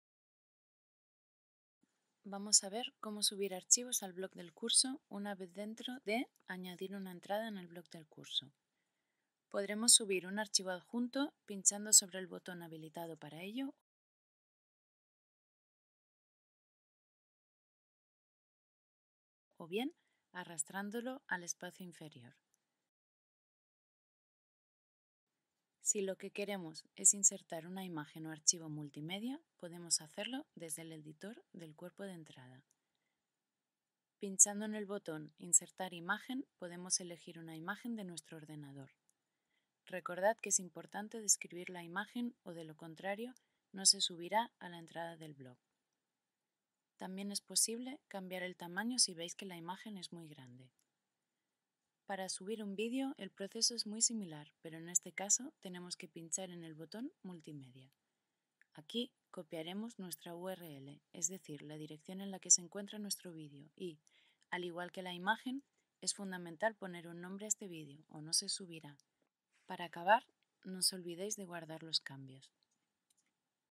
2 . Cómo subir archivos en el blog de los cursos tutorizados INTEF
Videotutorial para aprender a subir archivos a las entradas del blog de los cursos tutorizados INTEF.